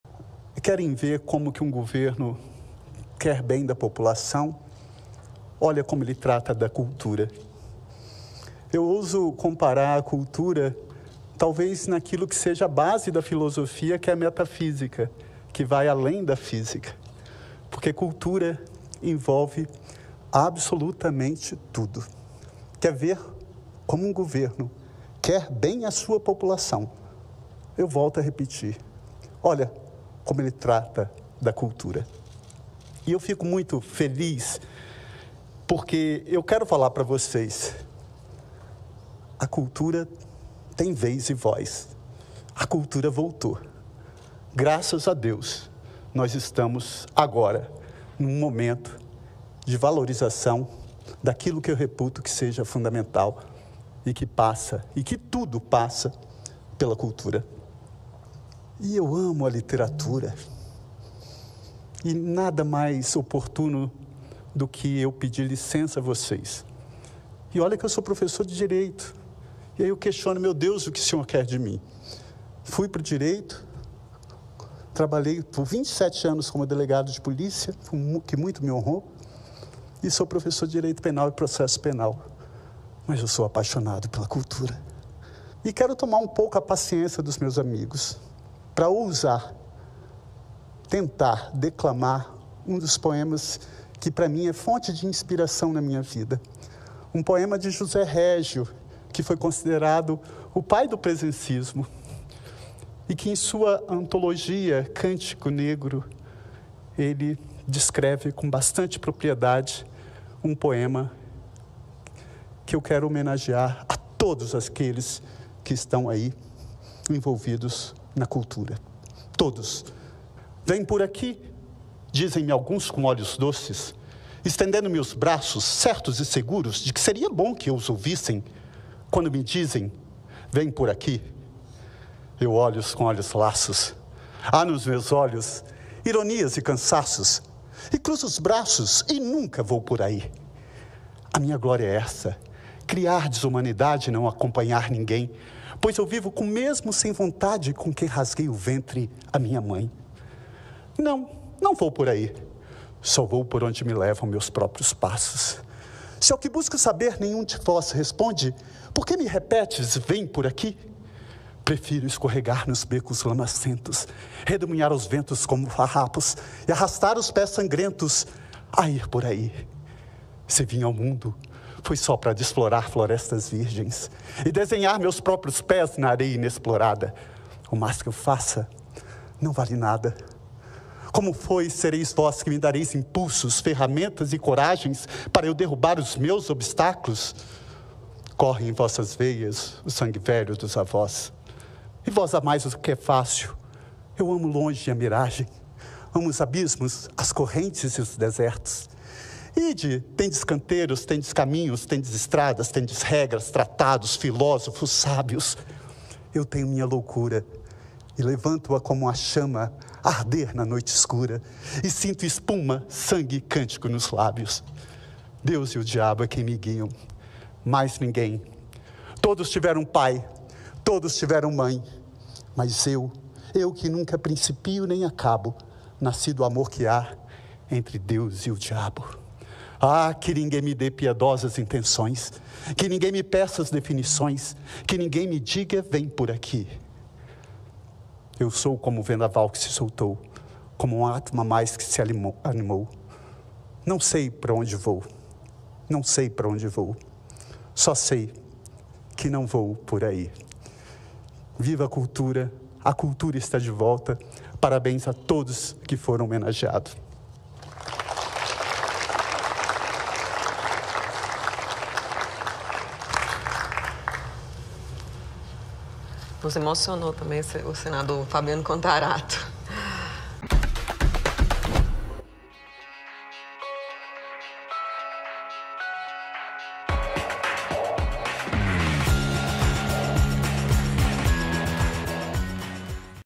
"O SENHOR NOS EMOCIONOU, SENADOR", DISSE A PRESIDENTE DA SESSÃO | Cortes 247
O senador Fabiano Contarato, líder do PT no Senado, emocionou a todos os presentes na Casa ao ler um poema do escritor e poeta português José Régio. Ele falou durante a entrega da Comenda de Incentivo à Cultura Luís da Câmara Cascudo, que este ano homenageou Flávio Capitulino, Milton Nascimento, Pedro Machado Mastrobuono, Yara Tupynambá e o Instituto Inhotim. Confira.